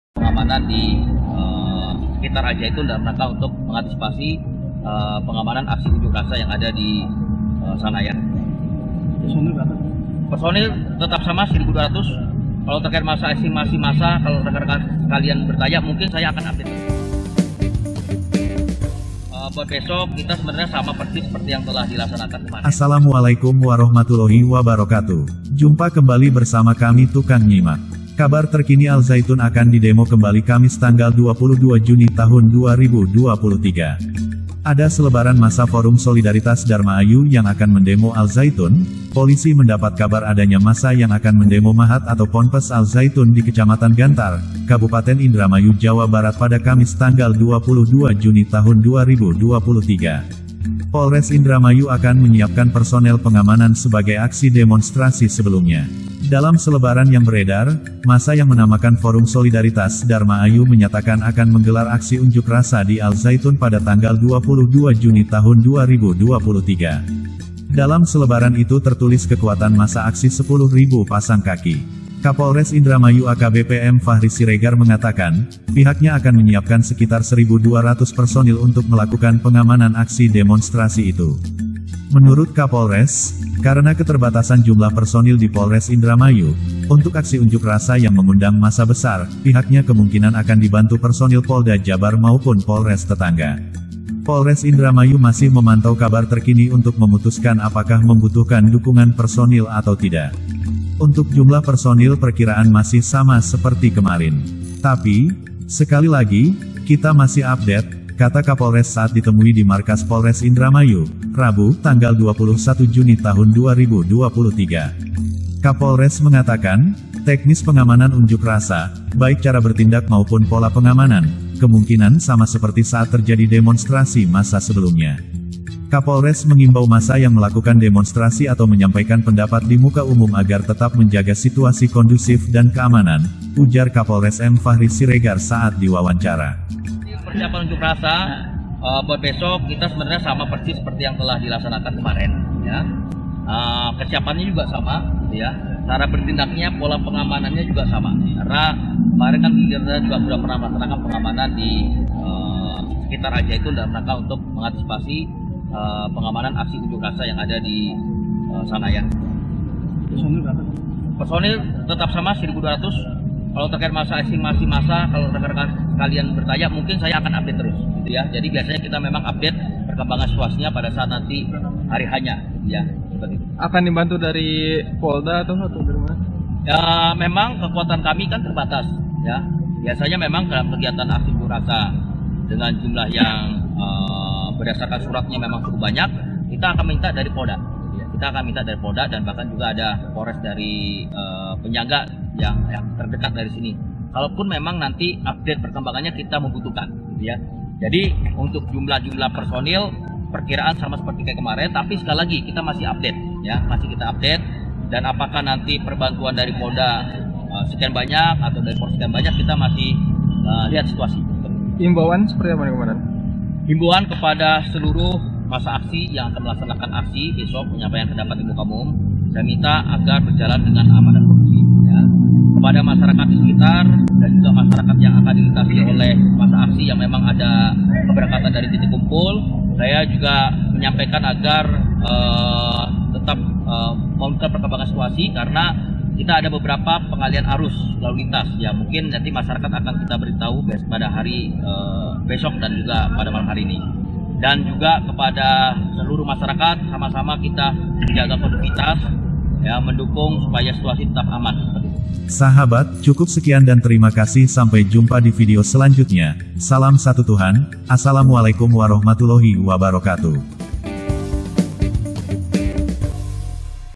PONDOK PESANTREN AL ZAYTUN KEMBALI DI DEMO KAMIS 22 JUNI 2023 | KAPOLRES INDRAMAYU SIAPKAN PERSONIL
Assalamualaikum ..
Salam Satu Tuhan !!
Terima kasih sahabat telah bergabung bersama kami Tukang Nyimak, untuk itu mari kita bersama sama membentengi umat dari pemurtadan !!

Kabar terkini Al Zaytun akan di demo kembali kamis 22/6/2023.
Ada selebaran massa Forum Solidaritas Dharma Ayu yang akan mendemo Al Zaytun, Polisi mendapat kabar adanya massa yang akan mendemo Ma’had atau Ponpes Al Zaytun di Kecamatan Gantar, Kabupaten Indramayu Jawa Barat pada Kamis 22/6/2023
Polres Indramayu akan menyiapkan personel pengamanan sebagai aksi demonstrasi sebelumnya.
Dalam selebaran yang beredar, massa yang menamakan Forum Solidaritas Dharma Ayu menyatakan akan menggelar aksi unjuk rasa di Al Zaytun pada 22 Juni 2023. Dalam selebaran itu tertulis kekuatan massa aksi 10 ribu pasang kaki.
Kapolres Indramayu AKBP M Fahri Siregar mengatakan, pihaknya akan menyiapkan sekitar 1.200 personil untuk melakukan pengamanan aksi demonstrasi itu.
Menurut Kapolres,karena ke terbatasan jumlah personil di Polres Indramayu, untuk aksi unjuk rasa yang mengundang massa besar, pihaknya kemungkinan akan dibantu personil Polda Jabar maupun polres tetangga.